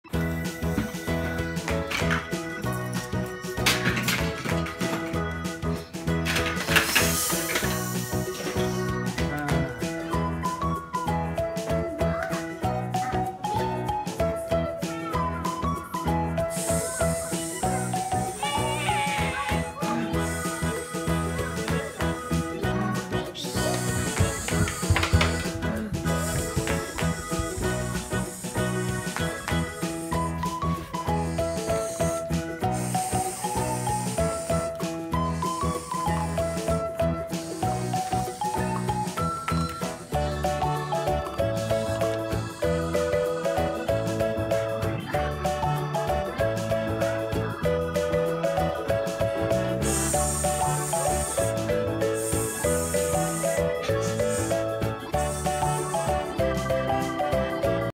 Toy snake attack
toy snake attacks kids